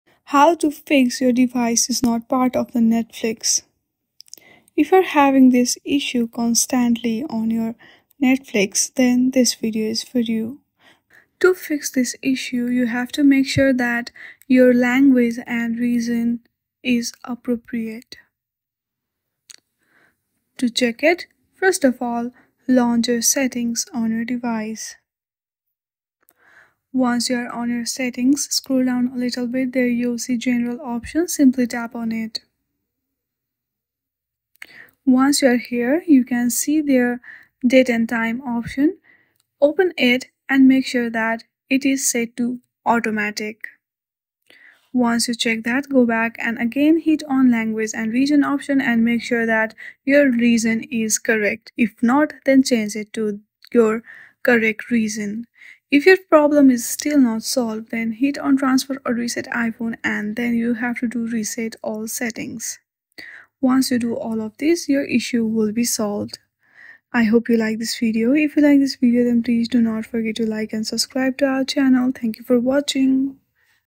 How to Fix Your Device is Not Part of the Netflix Household for this Account
Learn how to fix the issue where your device is not part of the Netflix household for this account in this step-by-step guide. Whether you're experiencing problems with device access or need to update your Netflix household settings, this tutorial will help you resolve the issue quickly and easily!

In this video, you'll discover how to address the "your device is not part of the Netflix household for this account" error. We'll walk you through the steps to fix the issue on both mobile and desktop devices.

Timestamps:
0:00 Introduction
0:12 Step 1: Understanding the Error Message
0:29 Step 2: Fixing Device Access Issues
0:55 Step 3: Updating Netflix Household Settings